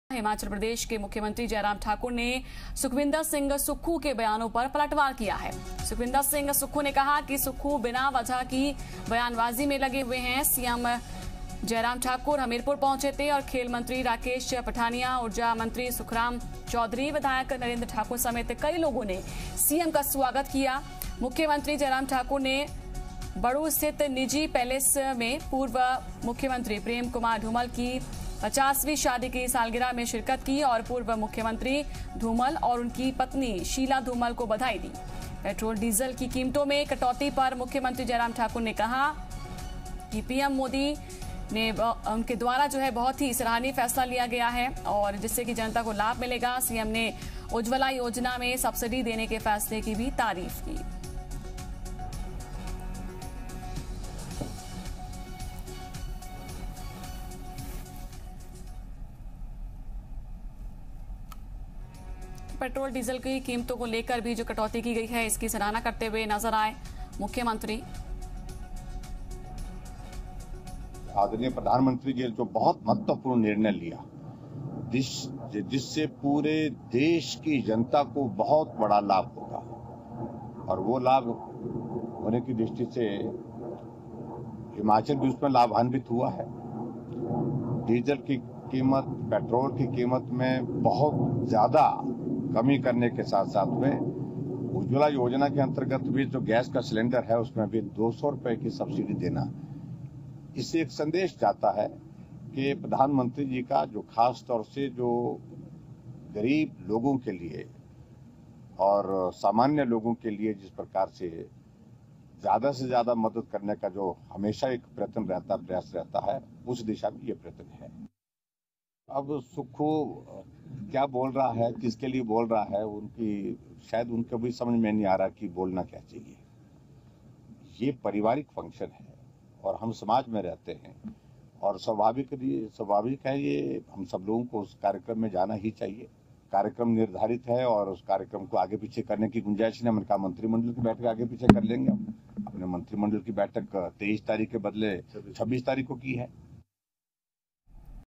CM Jairam Thakur का सुखविंदर सिंह सुक्खू के बयानों पर पलटवार | ZeePHH
CM जयराम ठाकुर का सुखविंदर सिंह सुक्खू के बयानों पर पलटवार
 सुक्खू बिना वजह की बयानबाजी में लगे हुए हैं- CM जयराम ठाकुर